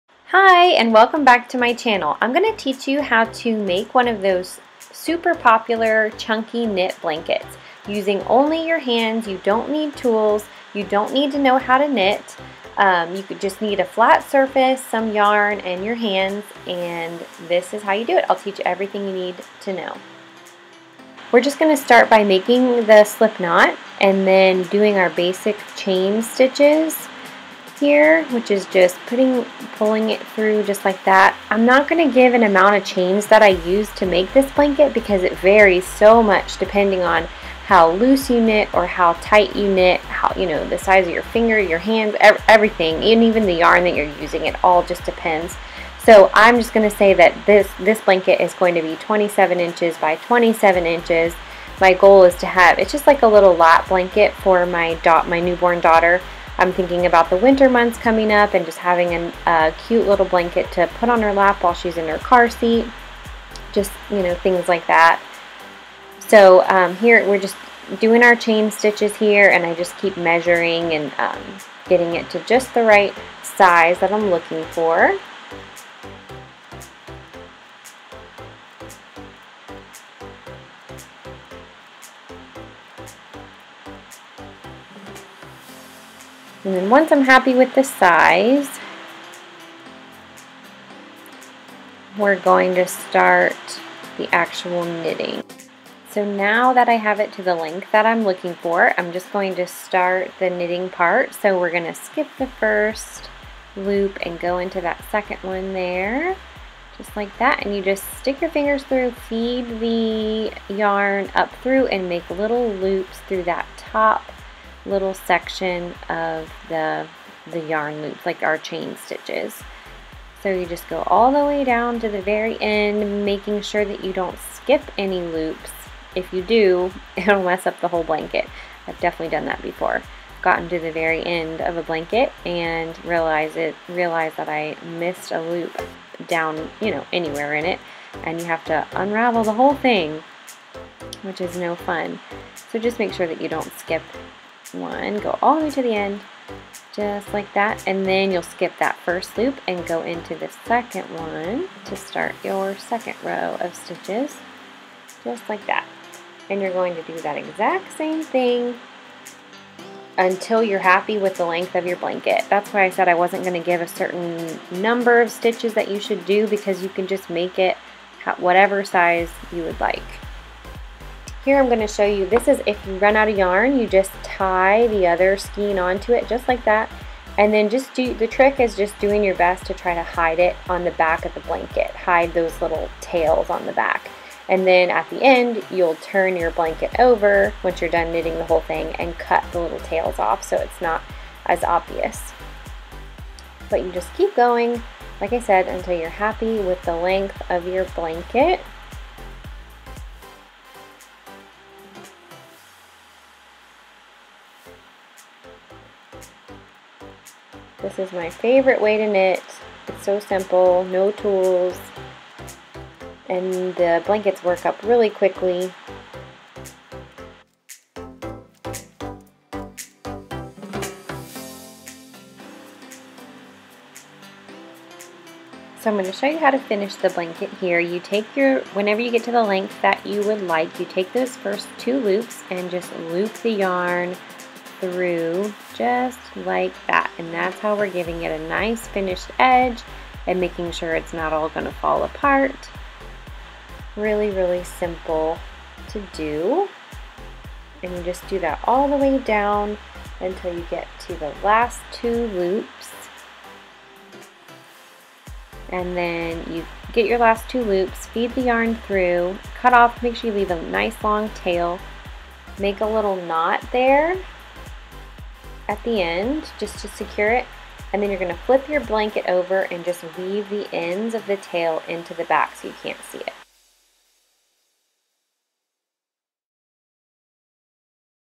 CHUNKY KNIT BLANKET | CHUNKY KNIT THROW | HAND KNIT CHUNKY BLANKET | DIY BLANKET
I know you have seen the incredibly stylish and sought after chunky knit blanket. The chunky knit throw has been super popular for years now and there is no end in sight to it's popularity. If you are loving the look of the chunky blanket, but not loving the price tag - look no further. In this tutorial, I am sharing how to hand knit chunky blanket on a budget - seriously!

Learn how to make this super simple chunky knit blanket using only your hands. No tools or knitting knowledge required!

If you are looking to buy a chunky knit blanket, I cannot recommend this blanket enough! It's made of the softest material, is oversized and doesn't shed!

My favorite yarns that work great for hand knitting:
My favorite for chunky knitting*
Another good one